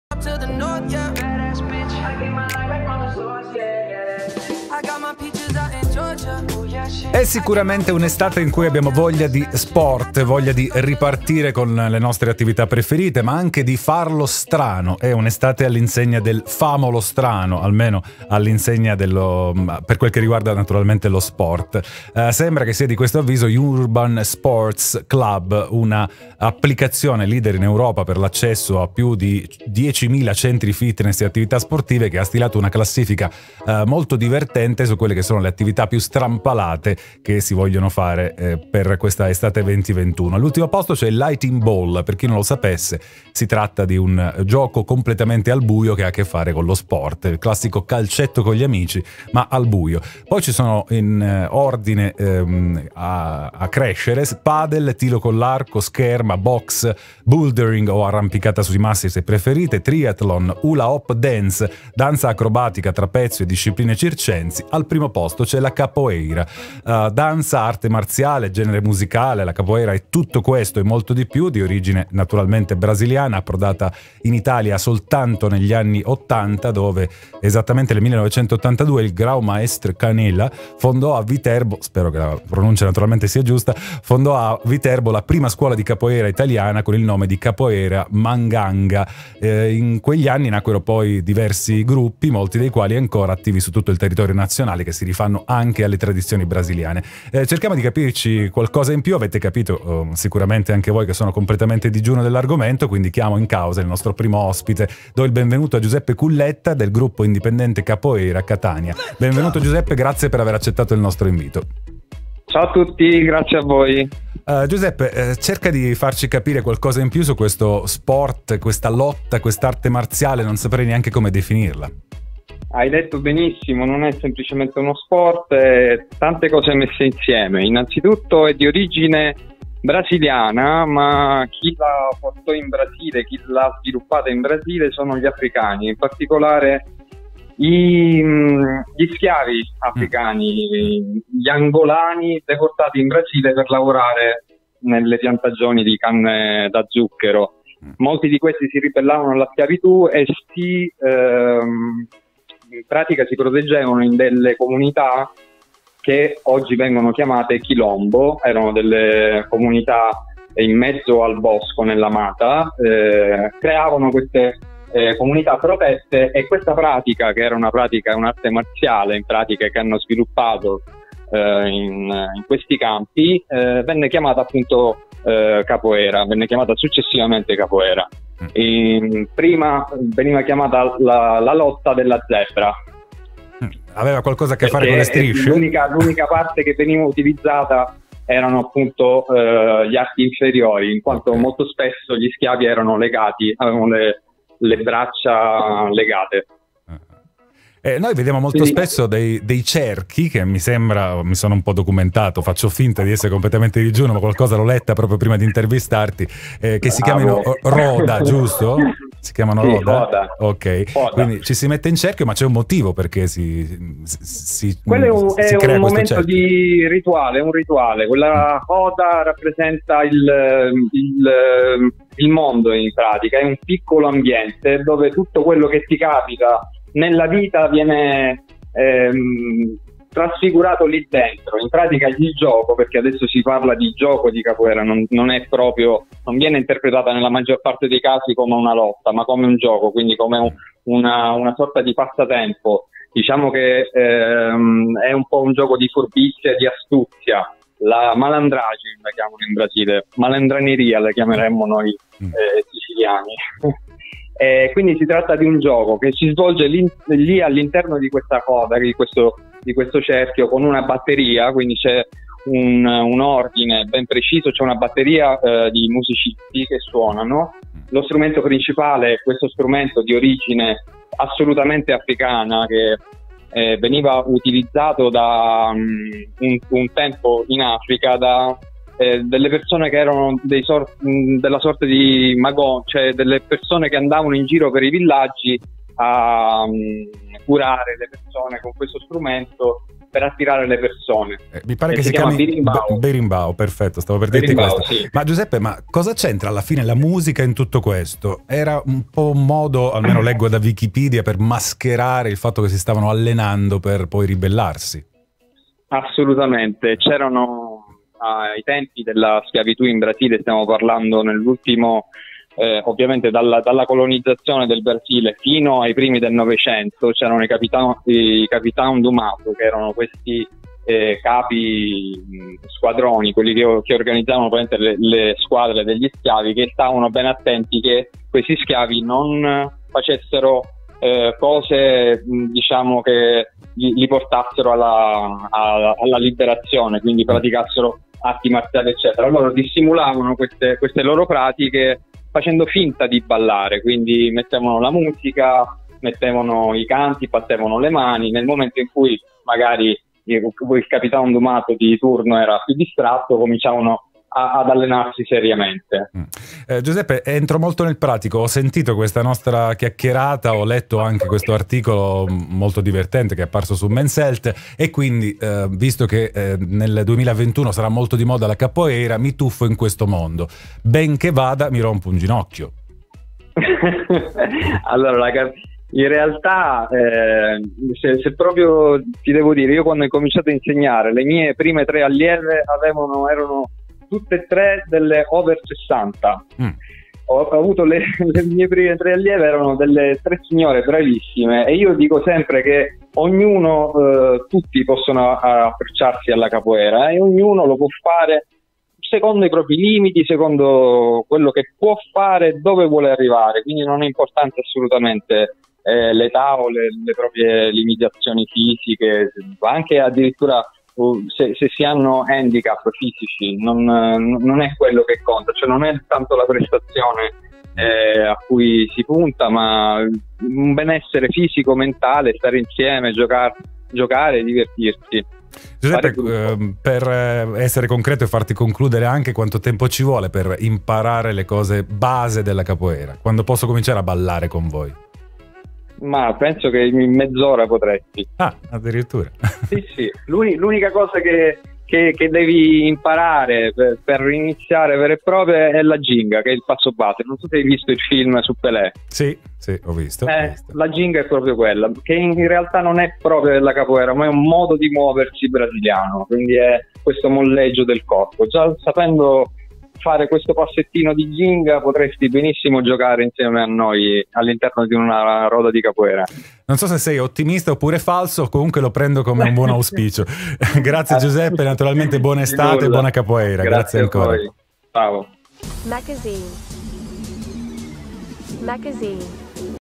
Estate e Capoeira
Giuseppe Culletta,del Gruppo indipendente Capoeira Catania, ha parlato con me dello "sport" più cool dell' estate 2021, la Capoeira.